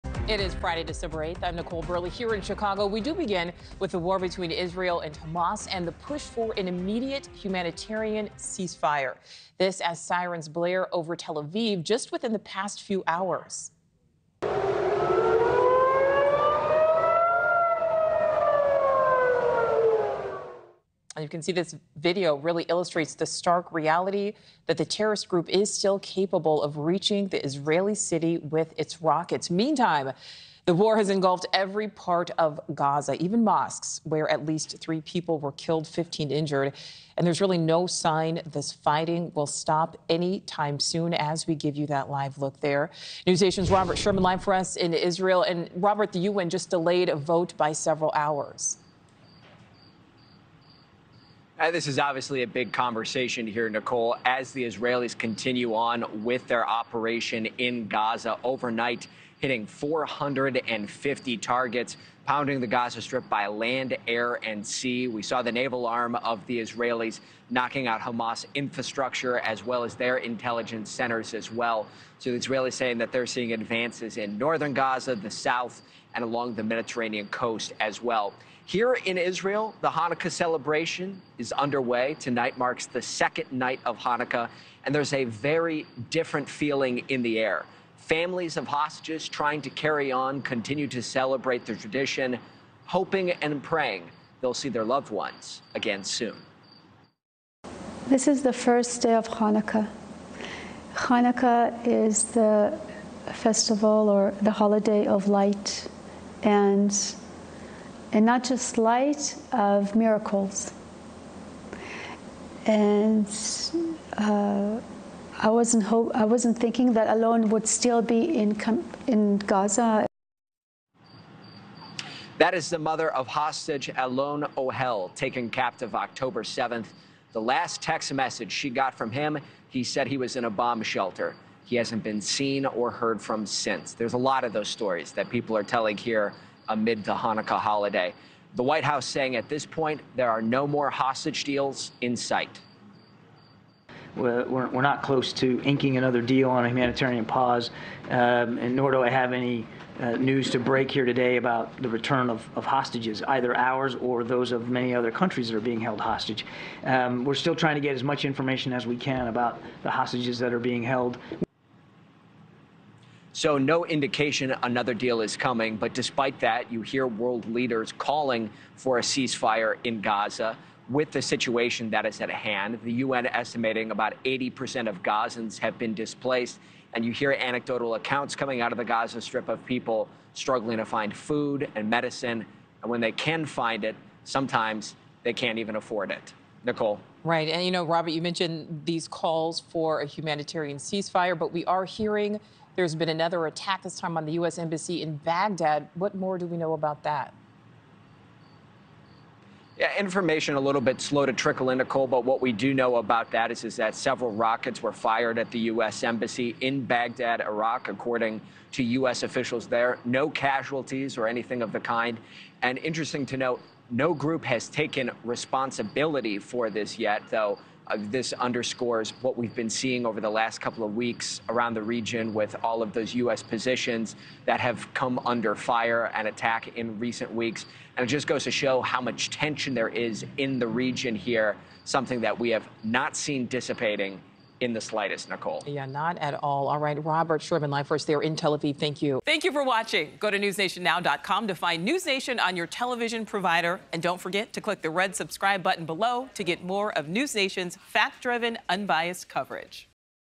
Israel strikes hundreds of targets | NewsNation Now
Israel struck hundreds of targets in Gaza overnight as sirens went off in Israel, warning of rockets on the second night of Hanukkah. While families of hostages pray for a miracle, the U.N. is calling for a cease-fire.